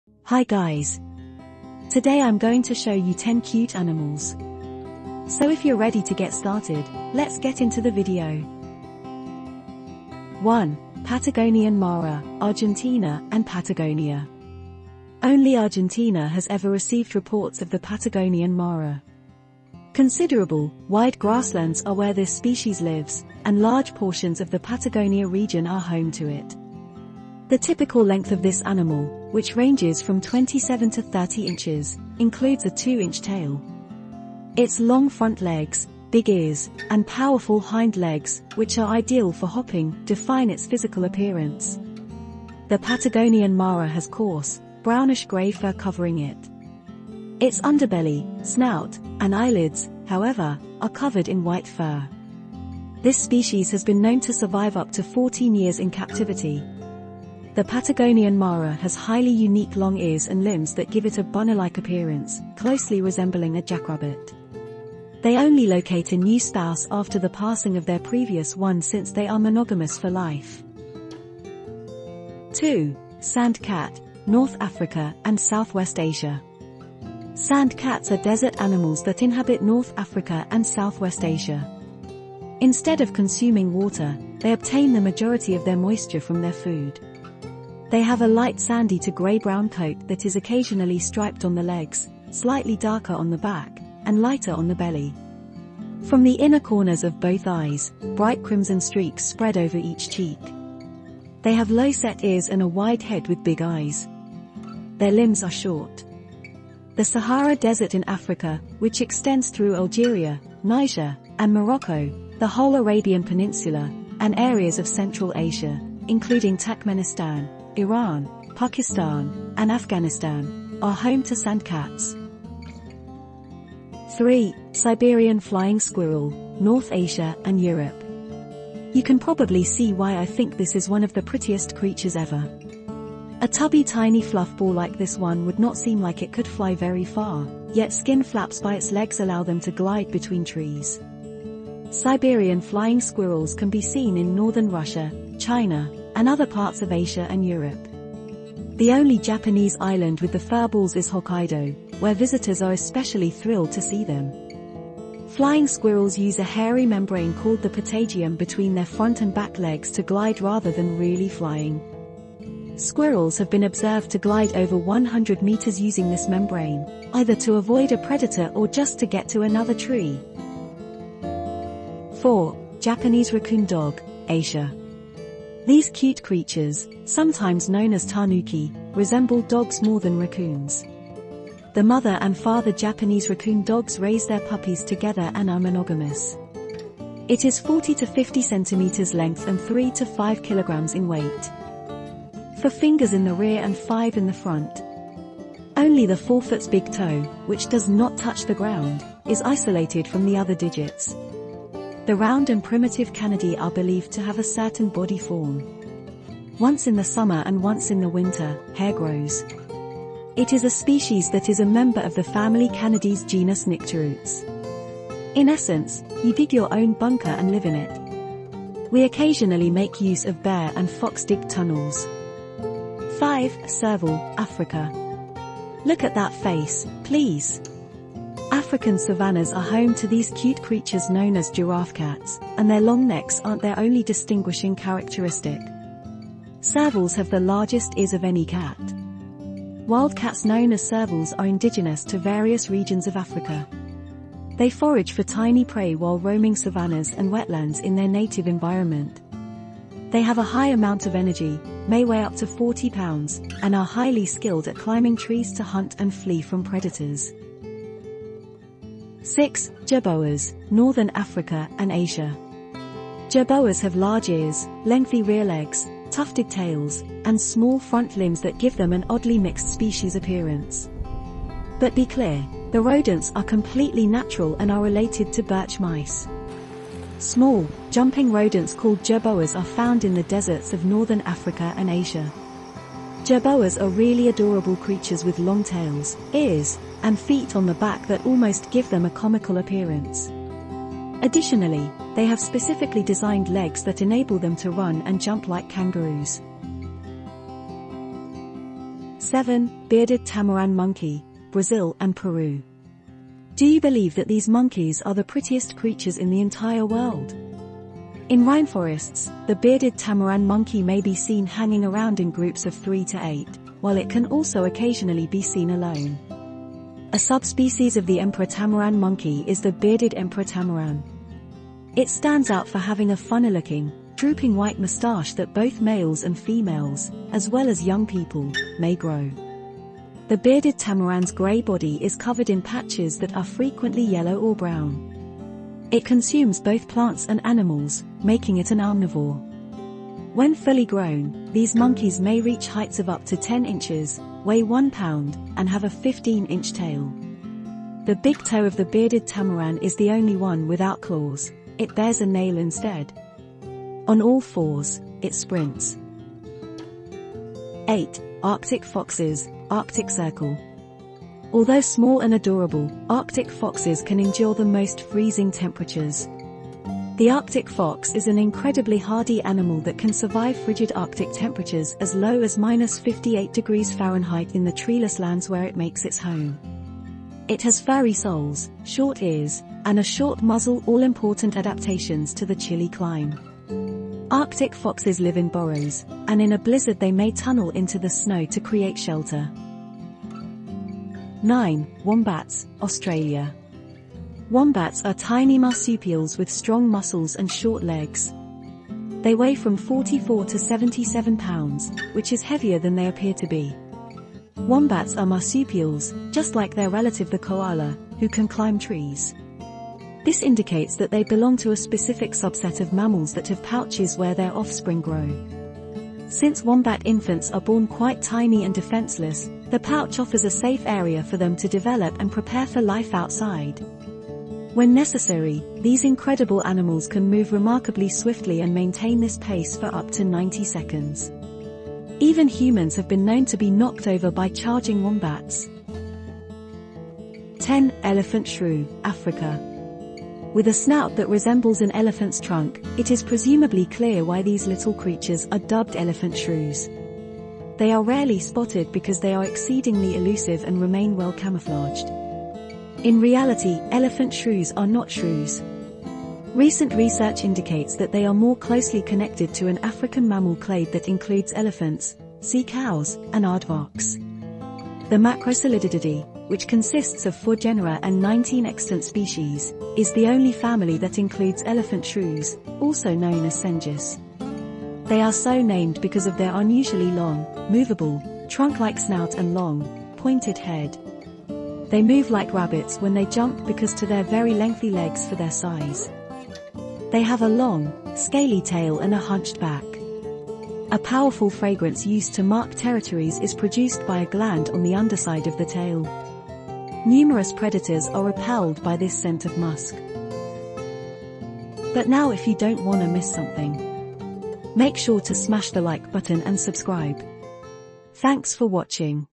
10 Cute Animals And Their Habitat
In this video i'm going to show you 10 cute animals and their
There are much more cute animals but for now those 10